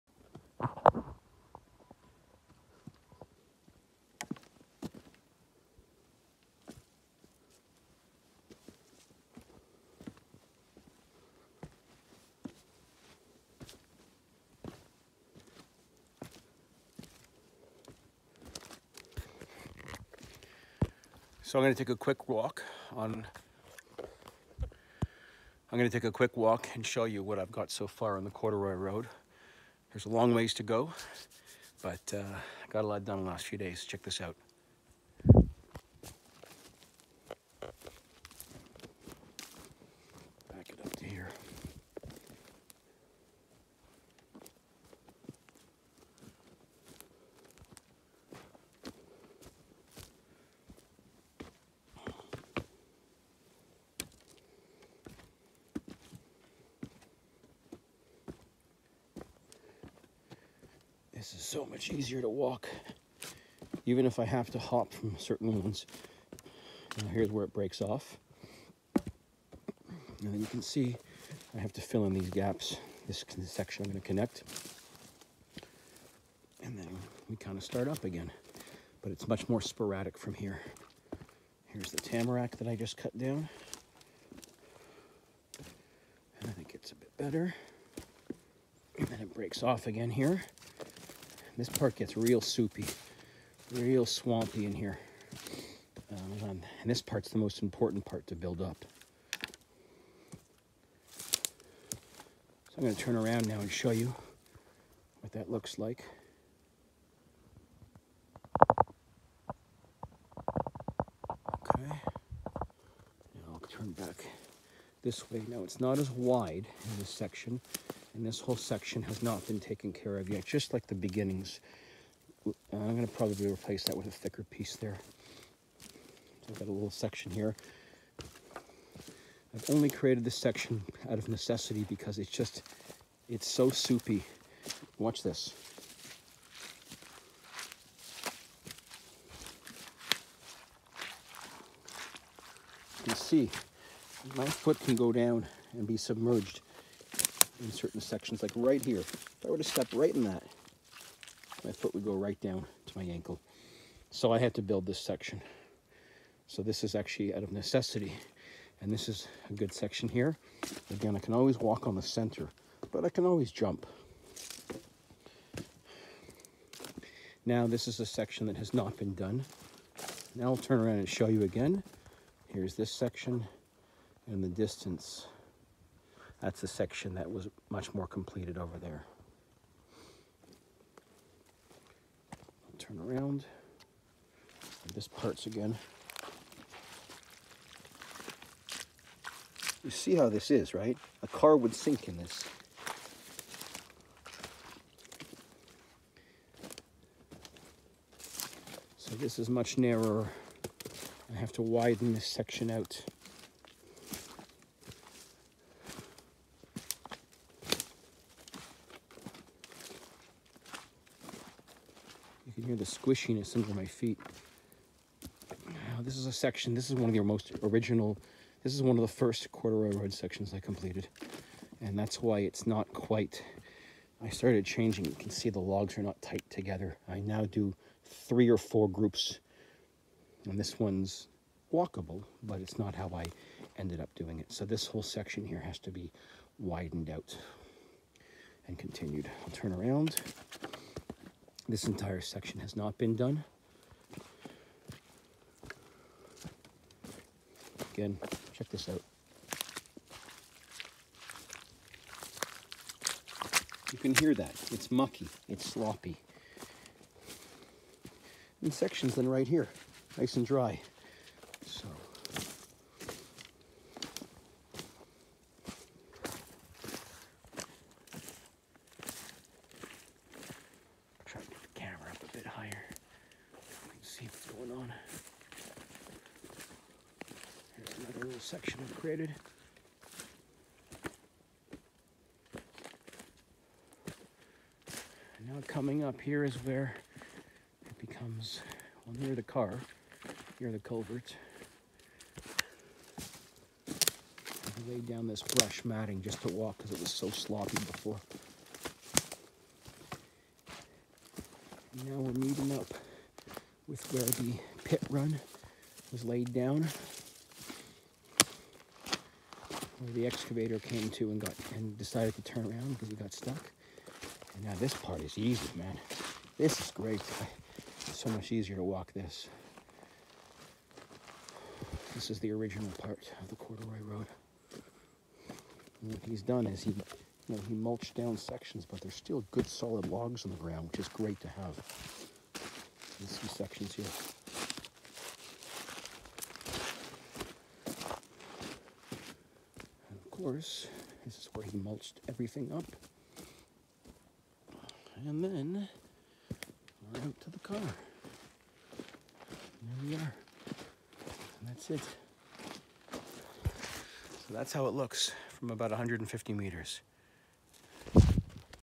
It’s So Much Easier To Walk With The Corduroy Road
This is an unedited walk through off the first 130m of the corduroy road.

This is the swampiest and least driveable section of the corduroy road but when I’m done with it, not only will it be a pleasure to walk but to use with an ATV and eventually my Ram 1500 4x4 or my AWD SUV

Part 2 (the edited version) is officially live! Watch it here. It looks really neat and you can see how it's all coming together. Its pretty amazing. Watch it and let me know what you think. Any suggestions are welcome.